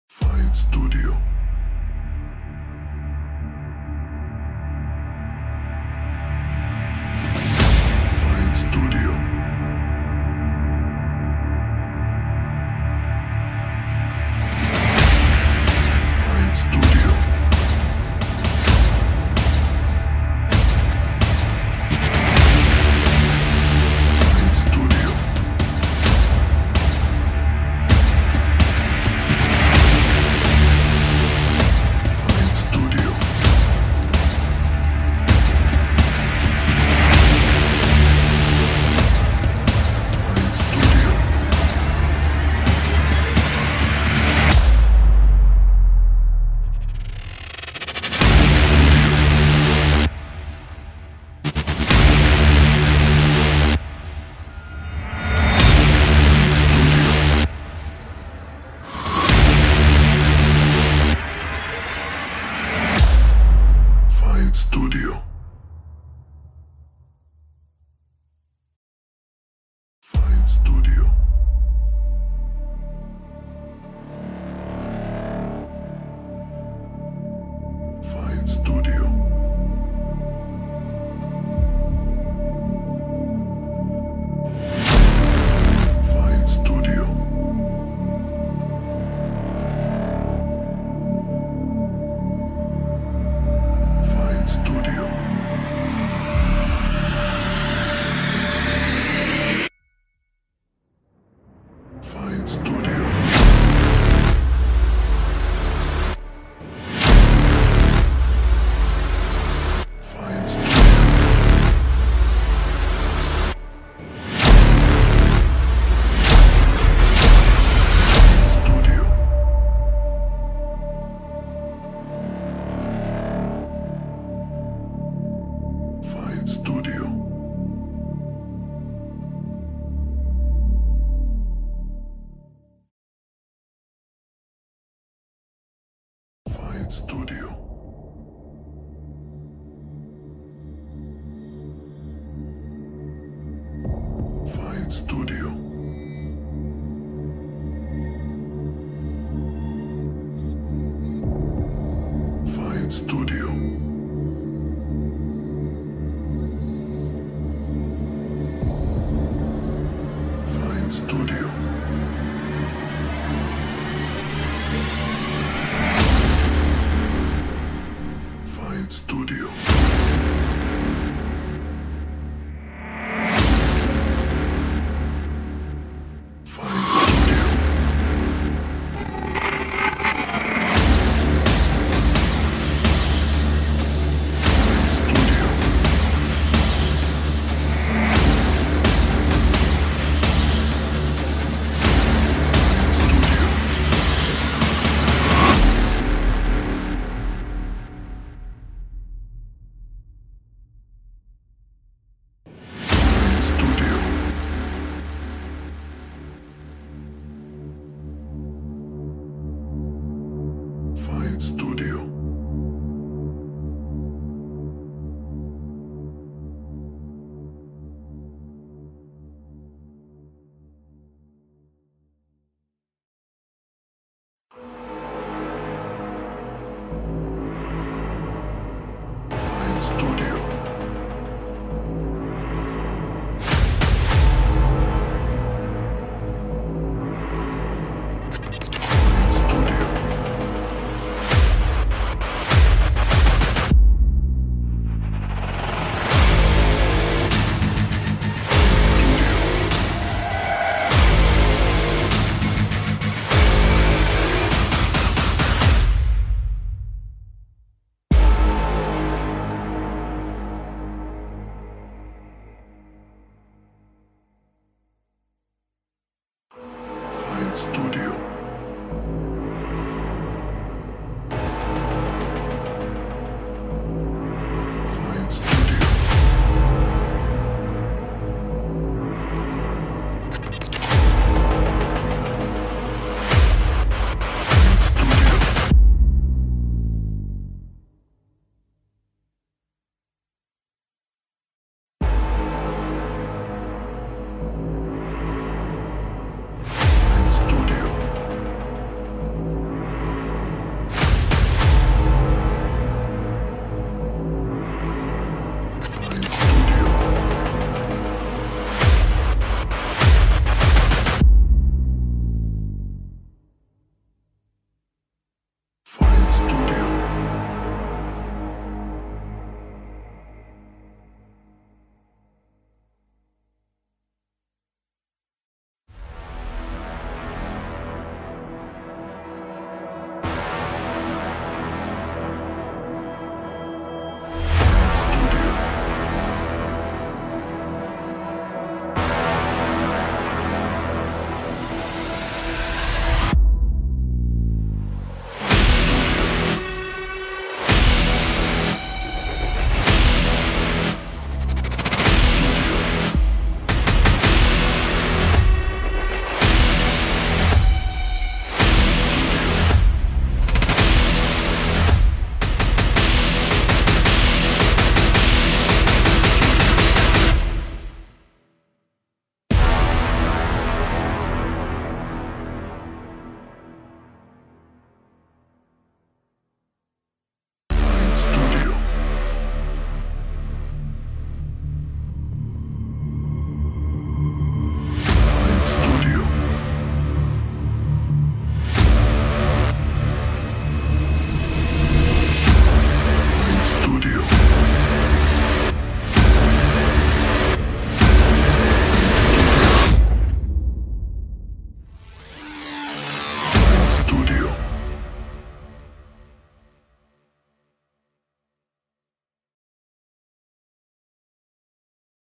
Top 6 Best of Movie Trailer Music
Movie Trailer Music is here to set new standards in terms of trailer .
6 Custom Made Music

Most Popular Music

1. Action Trailer Intro
2. Agressive Trailer
3. Agressive Trailer Intro
4. Epic Trailer Intro Ident ( 3 versions )
5. Trailer Intro
6. Trailer Intro Ident

Features:

6 Original Files
Channel : Stereo
Format : WAV+MP3
Resolution : 16 Bit / 44.1 kHz/ 320kbps

Ideal For Motion In:

Action
Trailers
Epic
Intro
Ident
Agressive

so once purchased, you can use them in your commercial releases without having to pay any hidden costs.